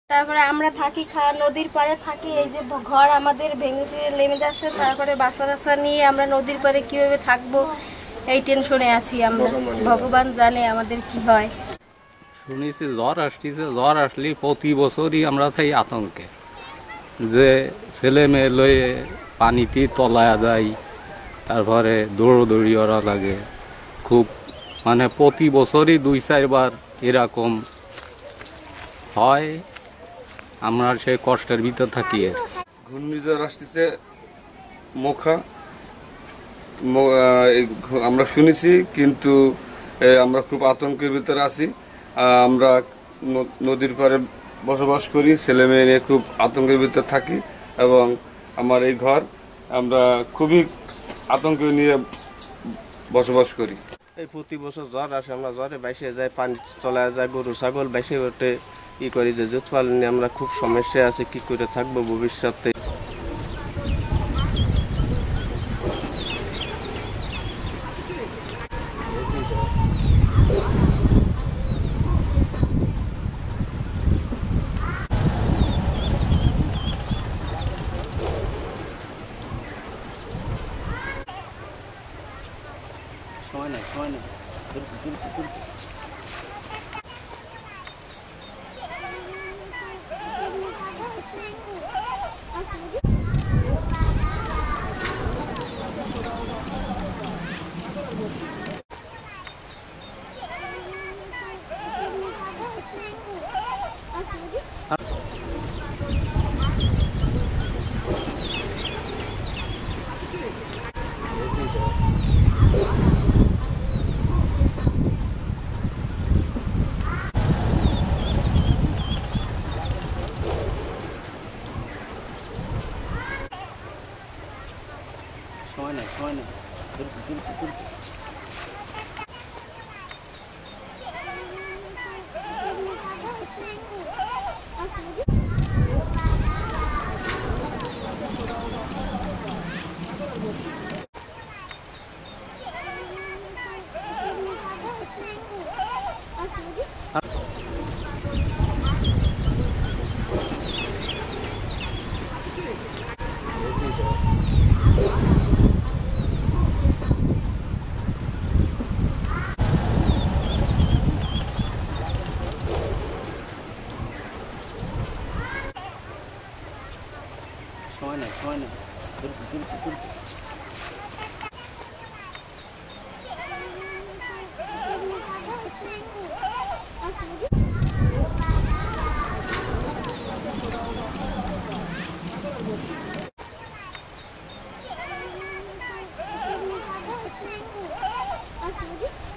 মোখা মোকাবিলায় প্রস্তুত বাগেরহাটে ৪৪৬ সাইক্লোন সেল্টার || Bagerhat || Deepto News
মোখা মোকাবিলায় প্রস্তুত বাগেরহাটে ৪৪৬ সাইক্লোন সেল্টার

Address:
Deepto TV
7/A/GA Tejgaon Industrial Area,